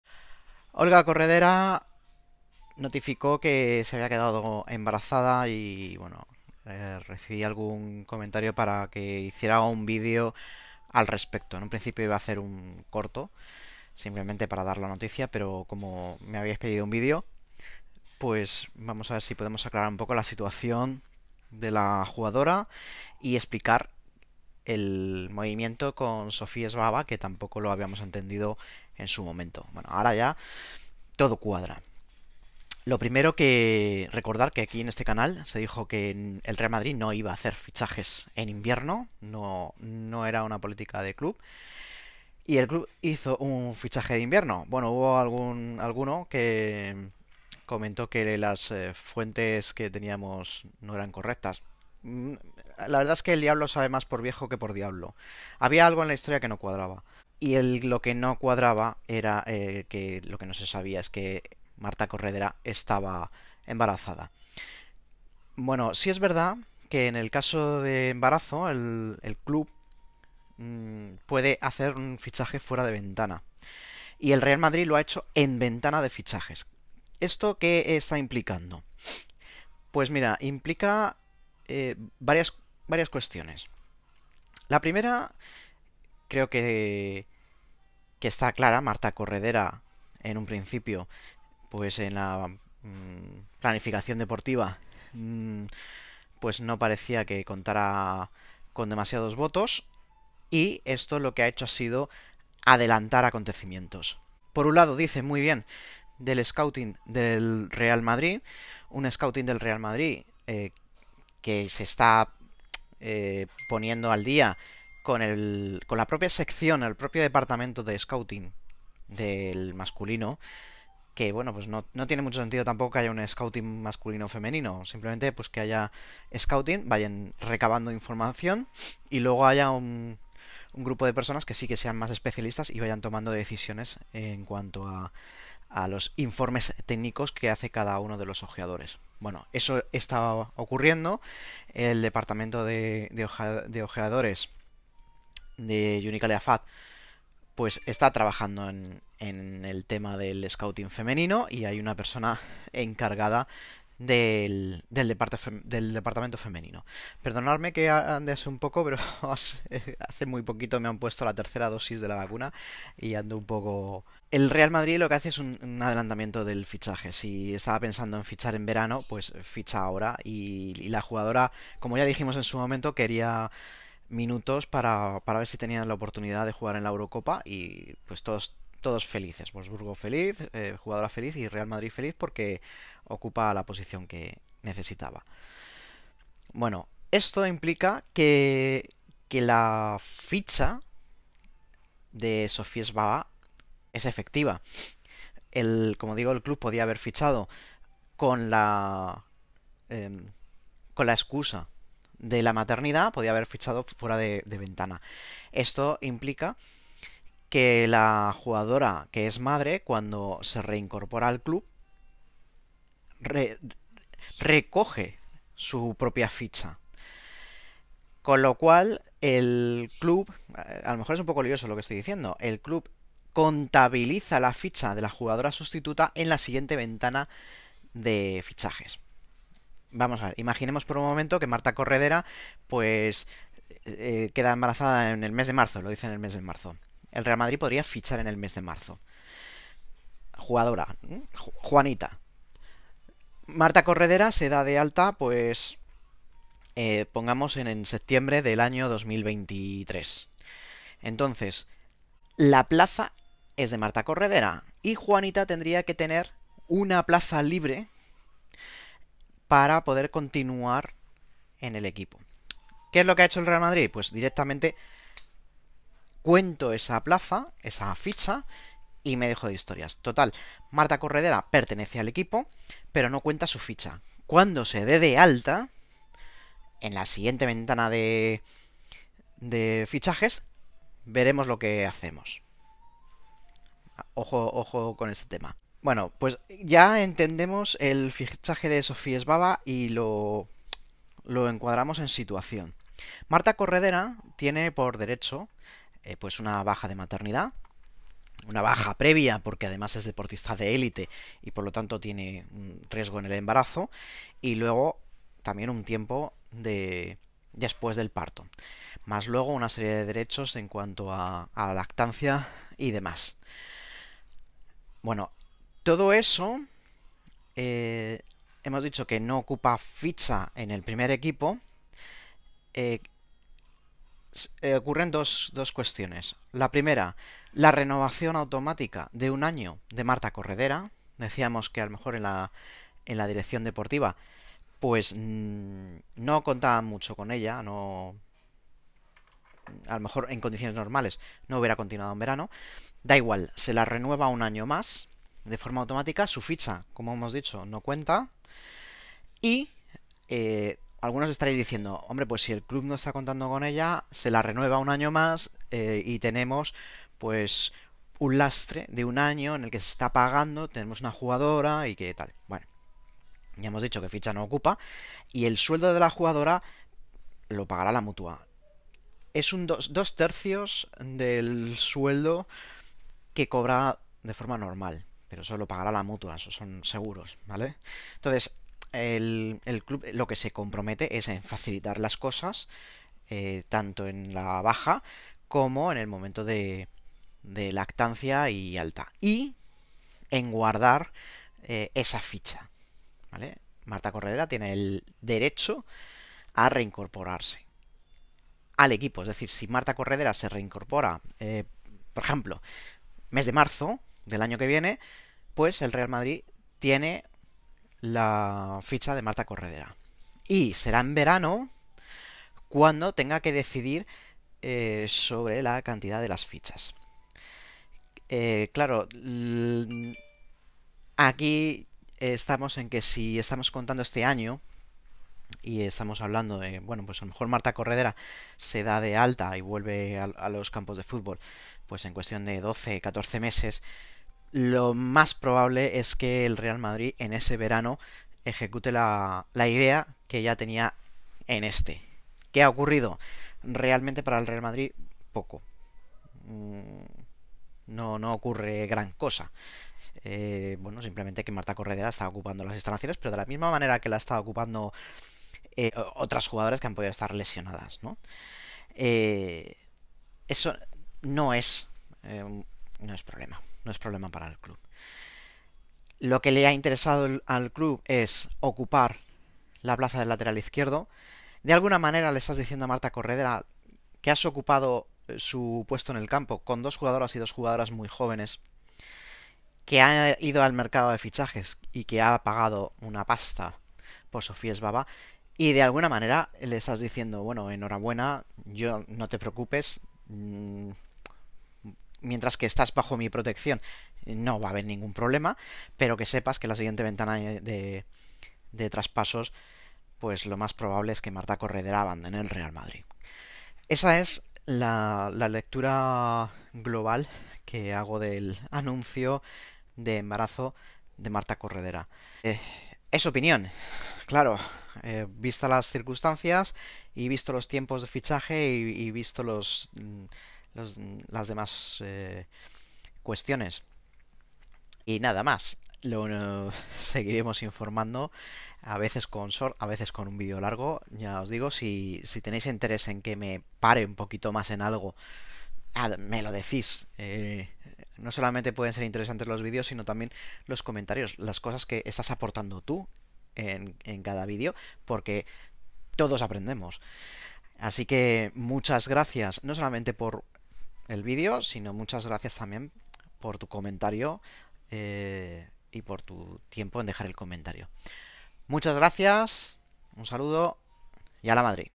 CORREDERA ¿Qué CONSECUENCIAS trae su embarazo?
El anuncio del embarazo, el fichaje de Sofie Svava... qué implica en el Real Madrid Femenino.